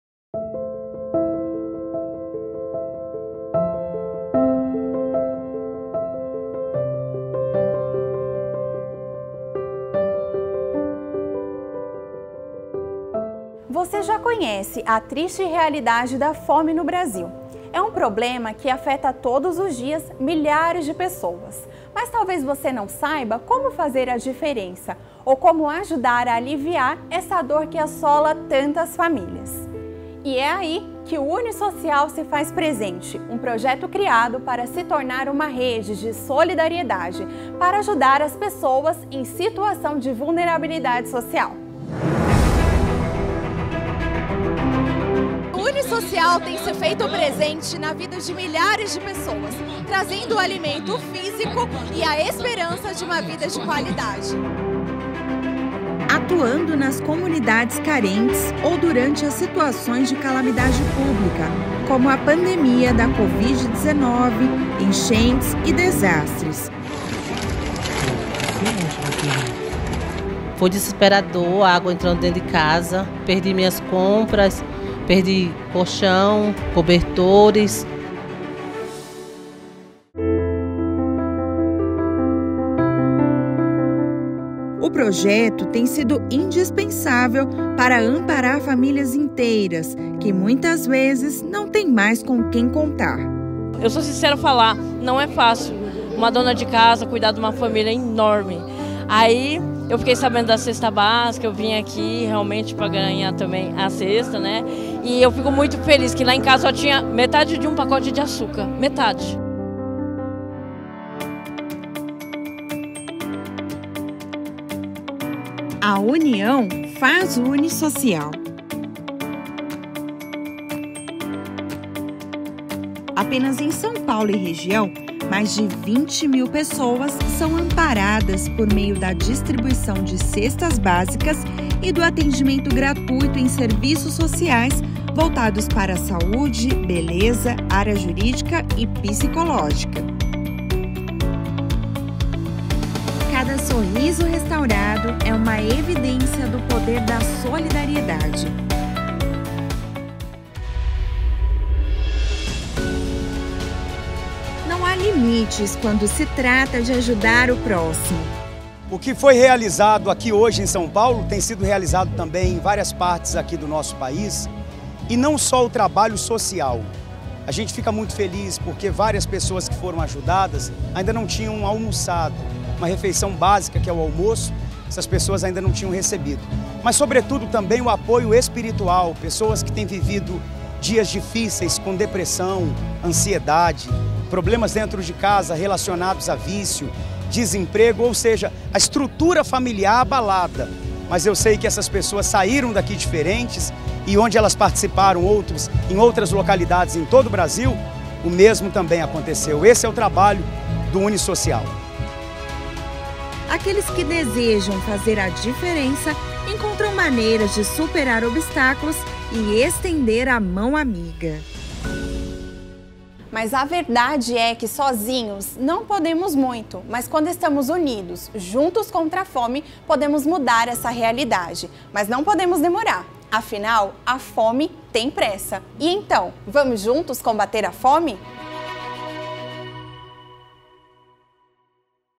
20 MIL PESSOAS AMPARADAS!
O Unisocial não tem sido indiferente às necessidades das pessoas. Mensalmente, há uma corrente de solidariedade voltada para arrecadar e distribuir alimentos às famílias carentes.

Iniciativas como a da Corrida Contra a Fome ajudam a intensificar ainda mais o trabalho, com a distribuição de milhares de cestas básicas.

Você já conhecia essas ações?